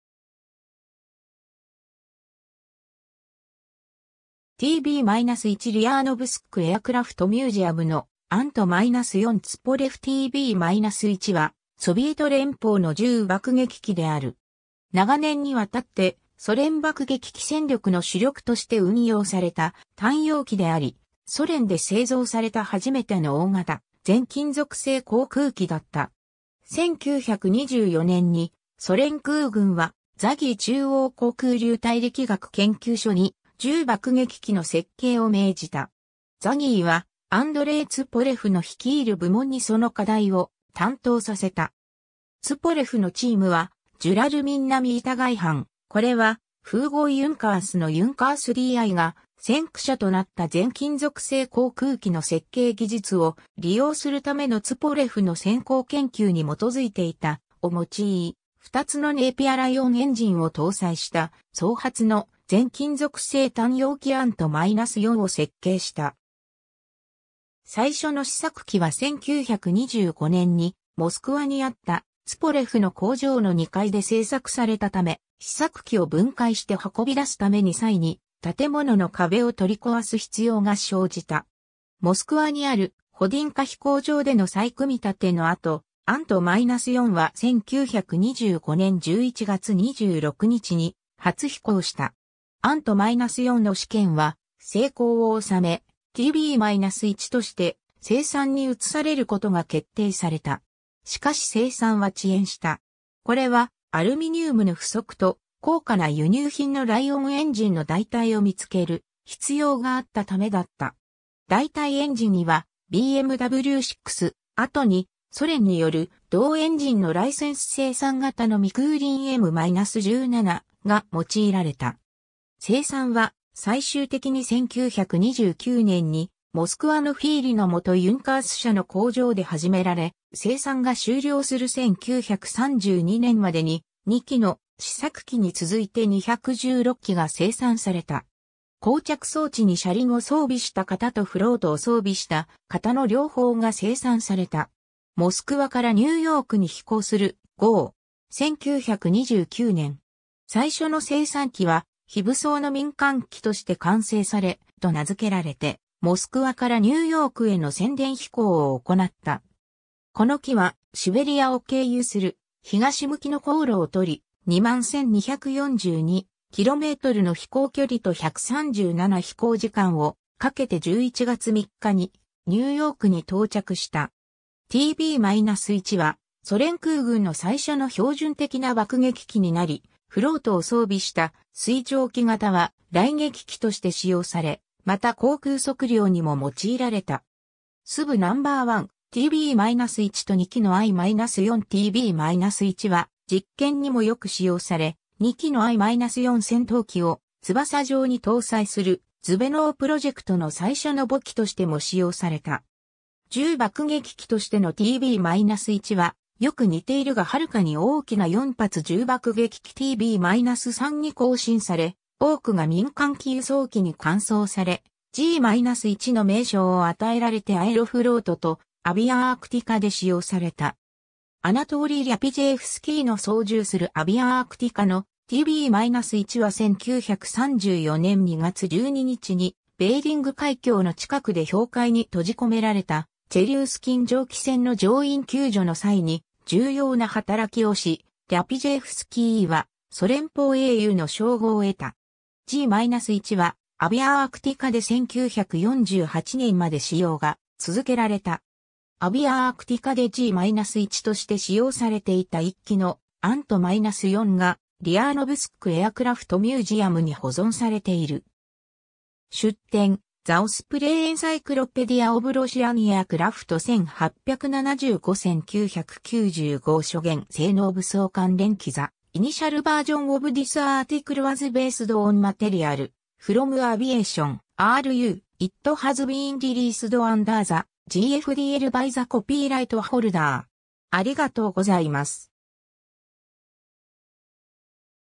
TB-1 (航空機・ソビエト連邦)
ソ連・ロシアの爆撃機
TB-1 Ulyanovsk Aircraft Museum のANT-4 ツポレフ TB-1 （開発名称ANT-4）はソビエト連邦の重爆撃機である。
長年に渡ってソ連爆撃機戦力の主力として運用された単葉機であり、ソ連で製造された初めての大型全金属製航空機だった。
 1924年にソ連空軍はTsAGI (Центра́льный аэрогидродинами́ческий институ́т (ЦАГИ) – 中央航空流体力学研究所) に重爆撃機の設計を命じた。
TsAGIはアンドレイ・ツポレフの率いる部門にその課題を担当させた。
 ツポレフのチームはジュラルミン波板外板―これはフーゴー・ユンカースのユンカース D.Iが先駆者となった全金属製航空機の設計技術を利用するためのツポレフの先行研究に基づいていた―を用い、2つのネイピア ライオンエンジンを搭載した双発の全金属製単葉機 ANT-4を設計した。
 最初の試作機は1925年にモスクワにあったツポレフの工場の2階で製作されたため、試作機を分解して運び出すために際に建物の壁を取り壊す必要が生じた。
モスクワにあるホディンカ飛行場での再組立ての後、ANT-4は1925年11月26日に初飛行した。
 ANT-4の試験は成功を収め、TB-1として生産に移されることが決定された。
しかし生産は遅延した。
これはアルミニウムの不足と、高価な輸入品のライオンエンジンの代替を見つける必要があったためだった。
代替エンジンにはBMW VI、後にソ連による同エンジンのライセンス生産型のミクーリン M-17が用いられた。
生産は最終的に1929年にモスクワのフィーリの元ユンカース社の工場で始められ、生産が終了する1932年までに、2機の試作機に続いて216機が生産された。
 降着装置に車輪を装備した型とフロートを装備した型の両方が生産された（ANT-4水上機は合計で66機が生産された）。
 モスクワからニューヨークに飛行する"Страна Советов"号。
1929年。
 最初の生産機は非武装の民間機として完成され、"Страна Советов"（ソビエトの土地）と名付けられてモスクワからニューヨークへの宣伝飛行を行った。
この機はシベリアを経由する東向きの航路をとり、21,242 kmの飛行距離と137飛行時間をかけて11月3日にニューヨークに到着した。
 TB-1はソ連空軍の最初の標準的な爆撃機になり、フロートを装備した水上機型は雷撃機(TB-1P)として使用され、また航空測量にも用いられた。
 Zveno-1: TB-1 と2機の I-4 TB-1は実験にもよく使用され、2機のI-4戦闘機を翼上に搭載するズヴェノー・プロジェクトの最初の母機としても使用された。
 重爆撃機としてのTB-1は、よく似ているがはるかに大きな4発重爆撃機 TB-3に更新され、多くが民間機輸送機に換装されG-1の名称を与えられてアエロフロートとアビアアークティカ（アエロフロートの極地部門）で使用された。
アナトーリィ・リャピヂェーフスキーの操縦するアビアアークティカのTB-1は1934年2月12日にベーリング海峡の近くで氷海に閉じ込められた チェリュースキン蒸気船の乗員救助の際に重要な働きをし、リャピヂェーフスキーはソ連邦英雄の称号を得た。
G-1はアビアアークティカで1948年まで使用が続けられた。
 アビアアークティカでG-1として使用されていた1機のANT-4がUlyanovsk Aircraft Museumに保存されている。
 出典: The Osprey Encyclopedia of Russian Aircraft 1875–1995 諸元 性能 武装 関連機 The initial version of this article was based on material from Aviation.ru. It has been released under the GFDL by the copyright holder.